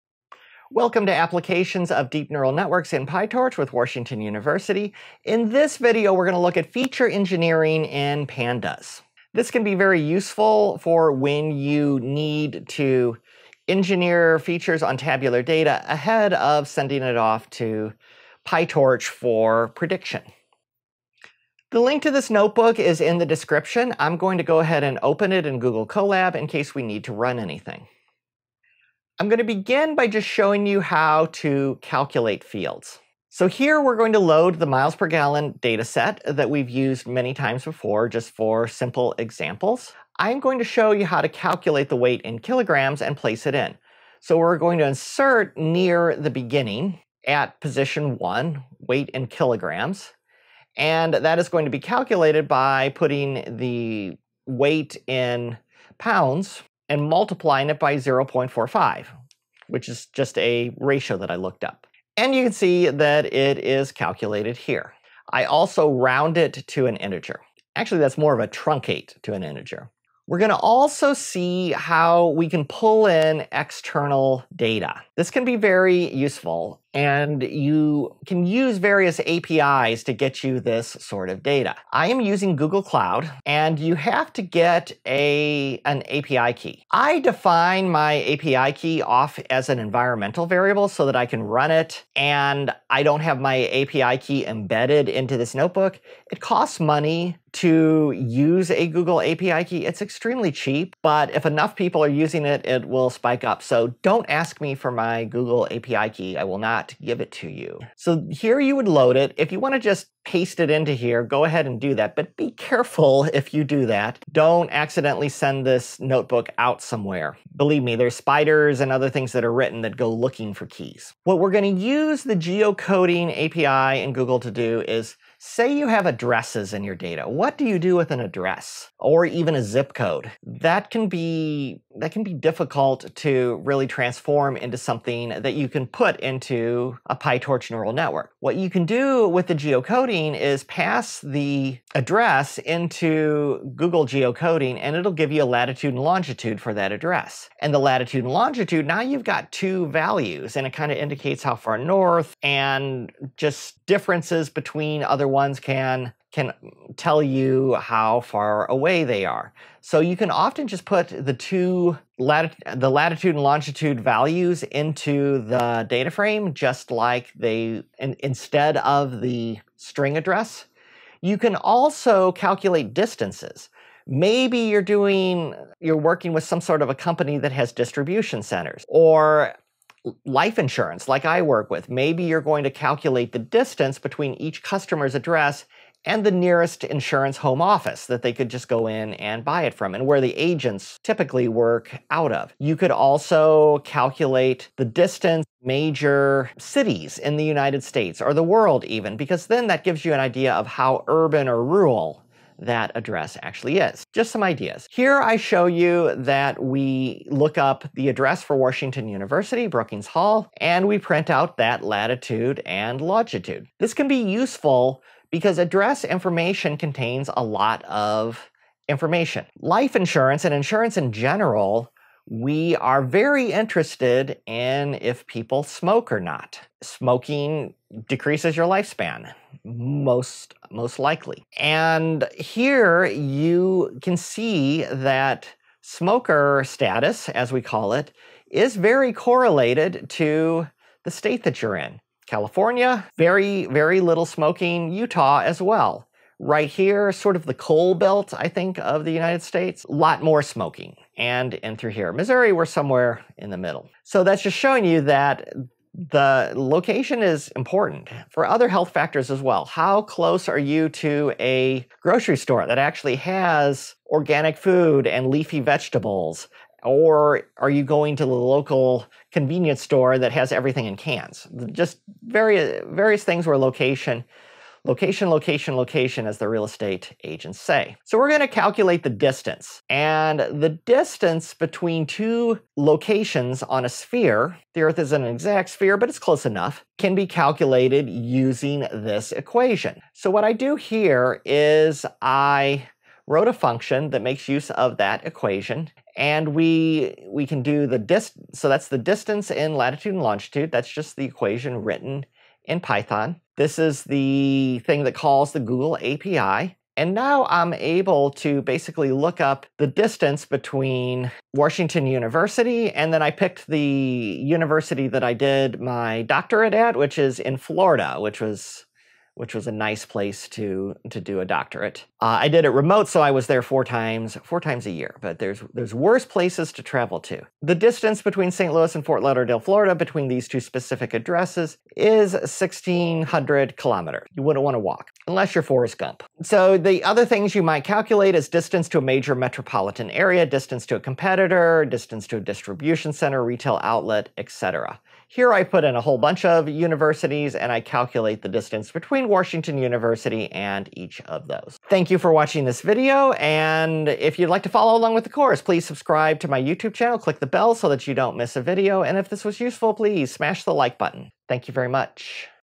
Feature Engineering in Pandas for Deep Learning in PyTorch (2.5)
Feature engineering is a critical skill for data science and deep learning.  This video shows how to use Pandas for feature engineering to prepare your data to maximize the predictive power of your machine learning models.

COURSE MATERIAL
📖 Textbook - Coming soon